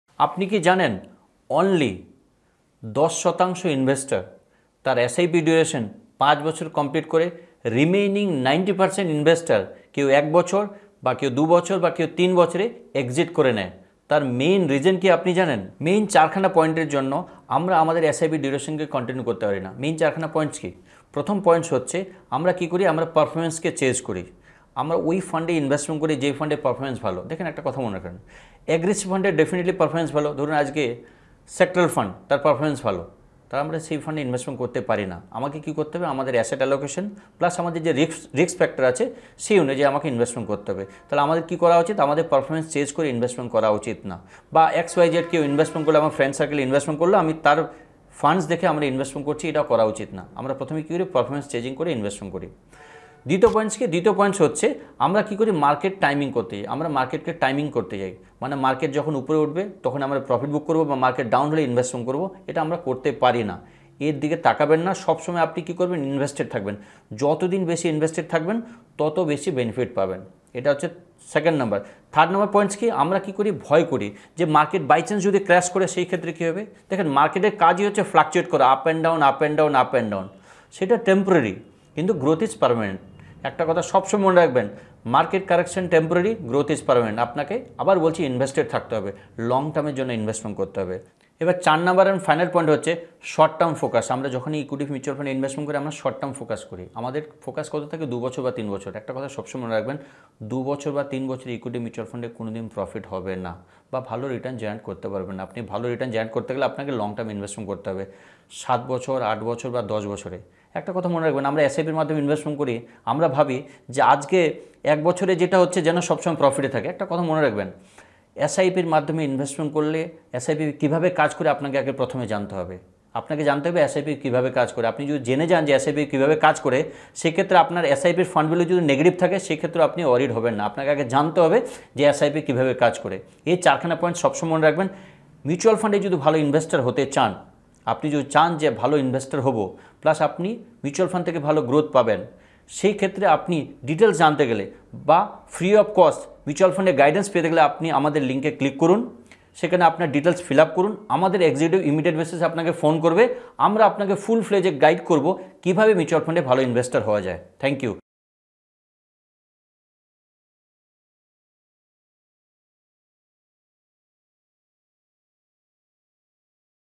How to avoid Investment Mistakes | Mutual Fund mistakes you must avoid | Bengali
Mistakes are part of the investing process. Knowing what they are, when you're committing them, and how to avoid them will help you succeed as an investor. To avoid committing the Mutual Fund investments mistakes above, develop a thoughtful, systematic plan, and stick with it. If you must do something risky, set aside some fun money that you are fully prepared to lose. Follow these guidelines, and you will be well on your way to building a portfolio that will provide many happy Mutual Funds returns over the long term. this is a bengali video on Mutual funds common mistakes in SIP, hope we can provide something useful to you. thank you.

✅✅✅ Free inquiry form for Mutual Fund Guidance | গাইডেন্সের জন্য বিনামূল্যে অনুসন্ধান ফর্ম